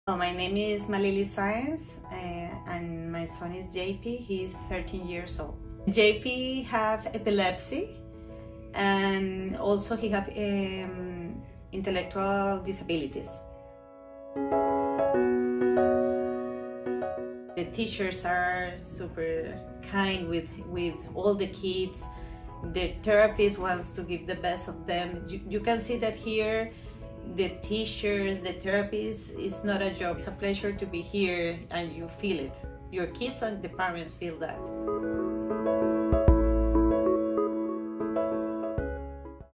Special Needs Parent Testimonial - Divine Savior School
Listen as one of our Divine Savior School parents share their experience.